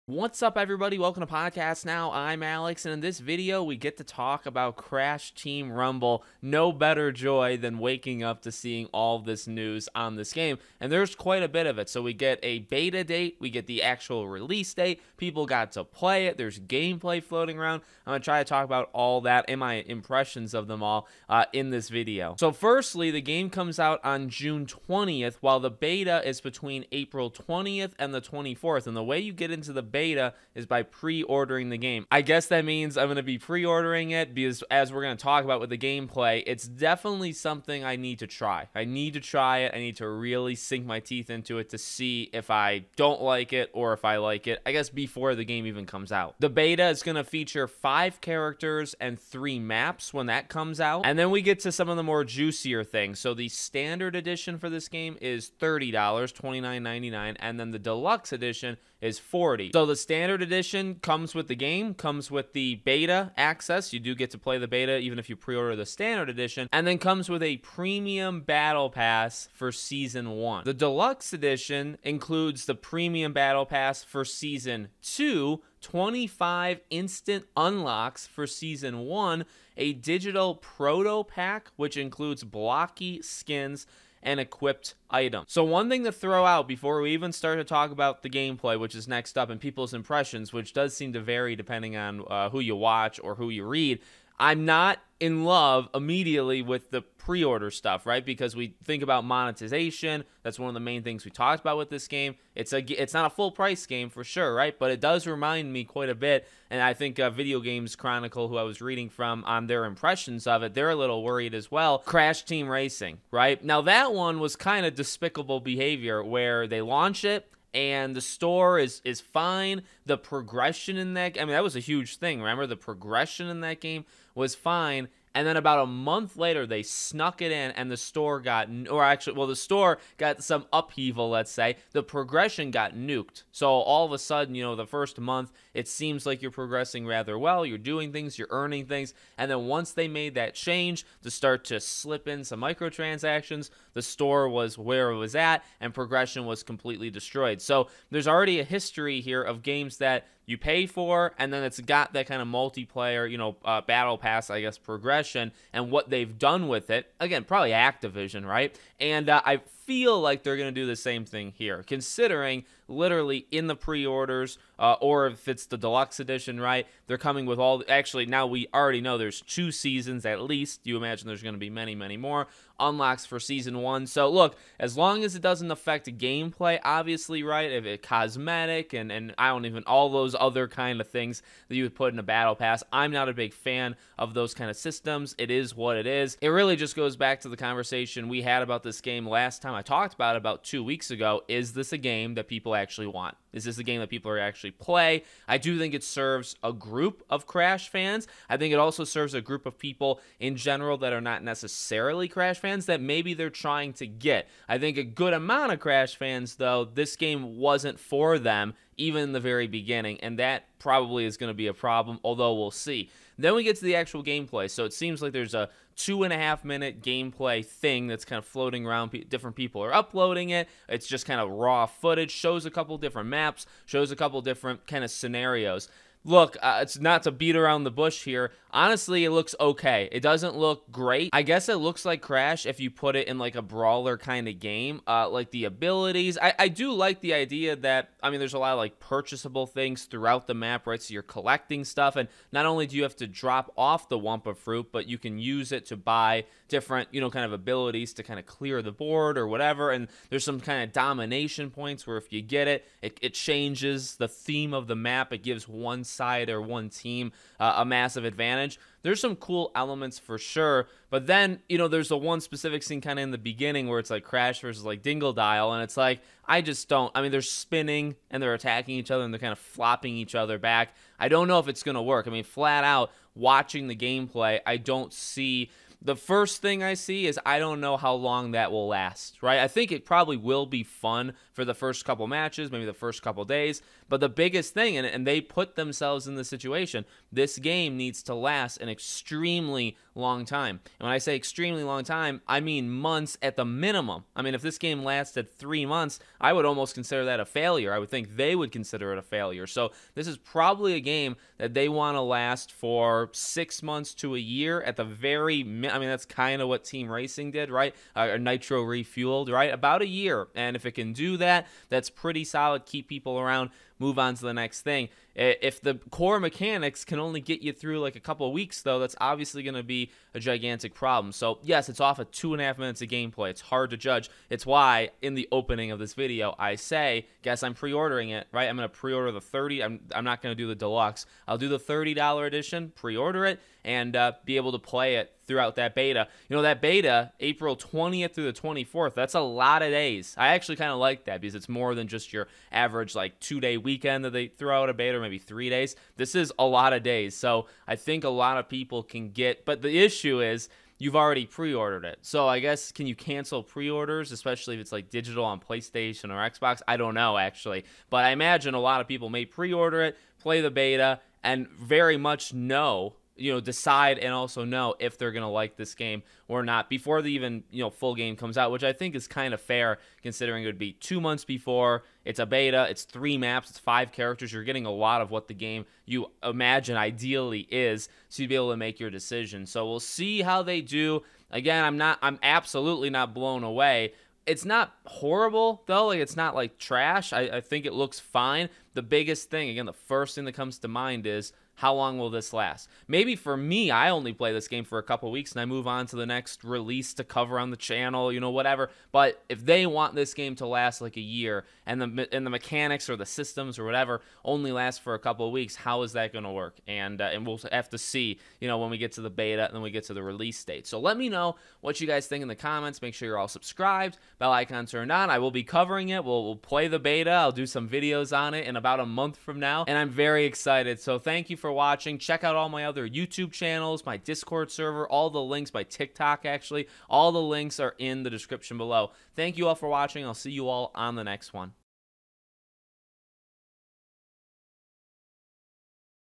Crash Team Rumble Release Date + NEW Details Revealed
Let's talk about the release date, the beta, and the new gameplay for Crash Team Rumble. It looks. interesting?